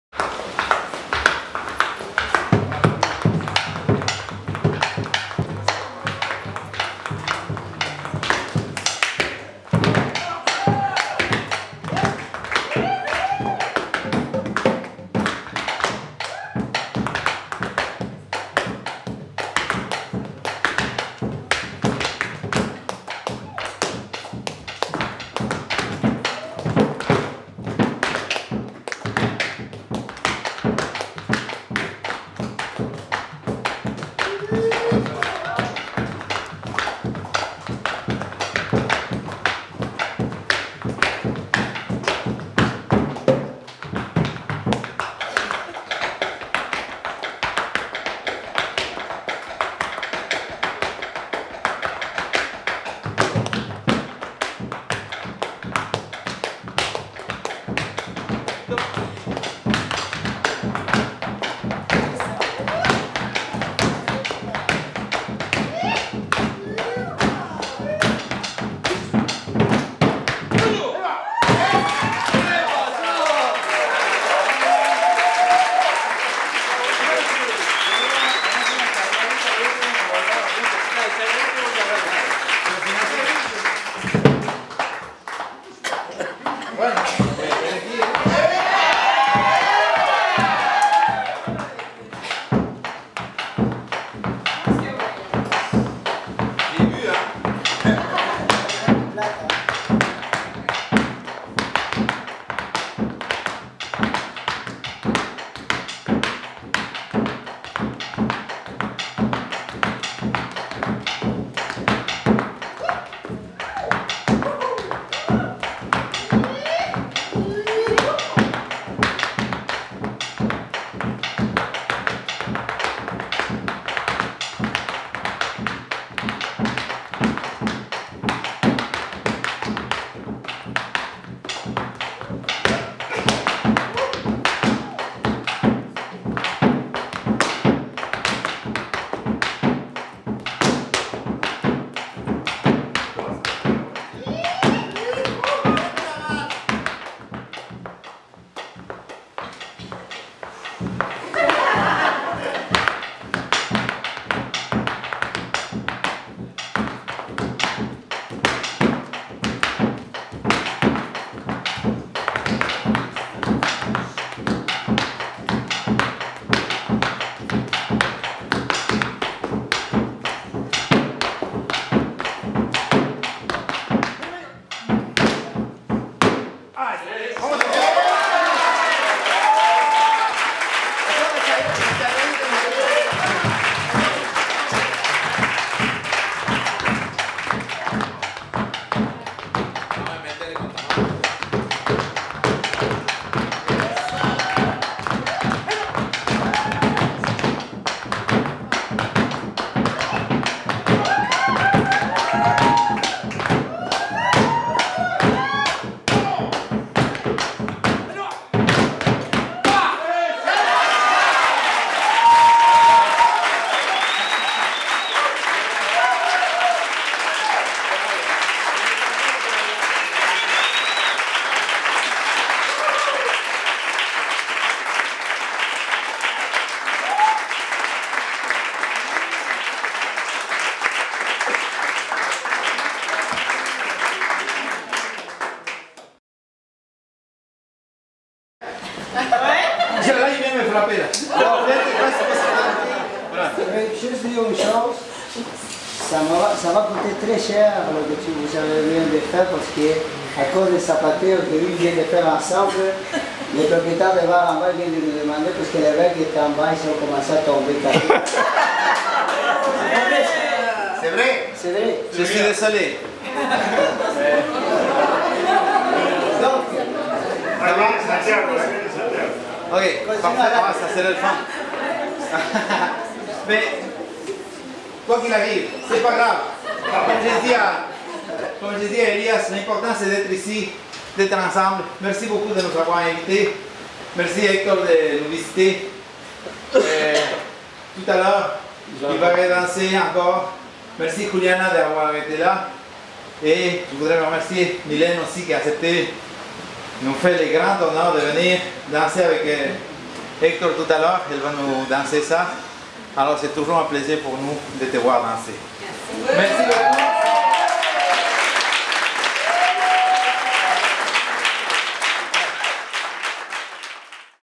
Hector Falcon, Luis Lopez, "Malambo".
Enregistré le 8 juin 2013, à l'école de tango argentin "Las Piernas", situé à Montréal (Québec).
Le malambo est une danse folklorique de l'Argentine, carartérisée par les percussions des pieds sur le sol.